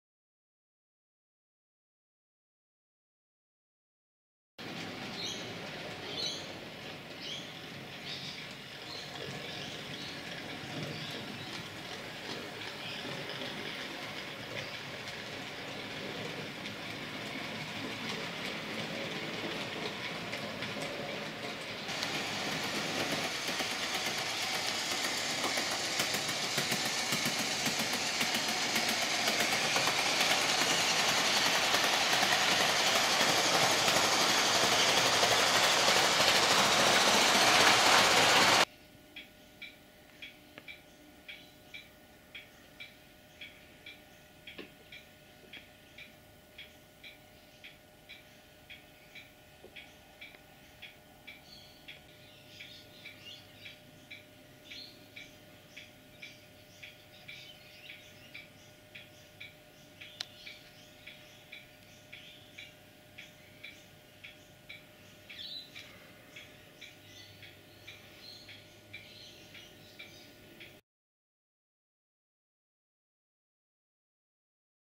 BAD ACCIDENT ON THE LAYOUT 3/11/2014
An inquiry is to be held regarding this accident on the layout.